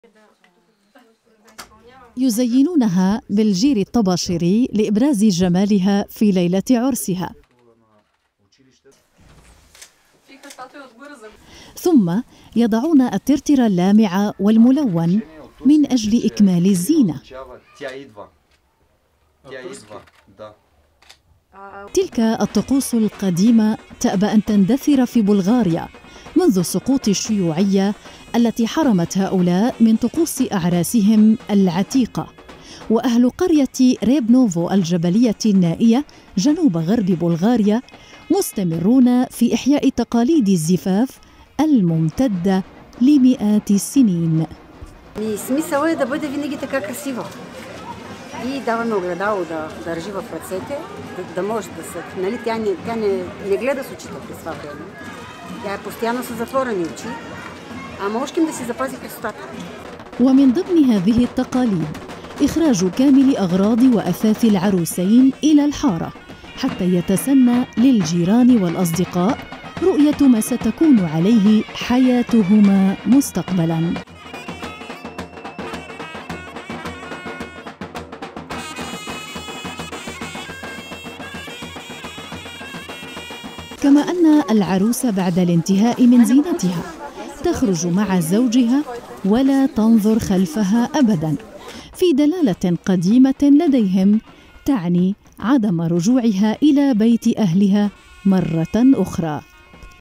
مكياج الجير الطباشيري في بلغاريا طقس قديم يعود لمئات السنين
مكياج الجير الطباشيري.. في بلغاريا تتزين العروس بالترتر الملون فوق طبقة سميكة من الجير الطباشيري في طقس قديم يعود لمئات السنين
لمعرفة أحدث المستجدات.. عبر البث المباشر من هنا ⤵
وبإمكانكم متابعة العربية على منصات التواصل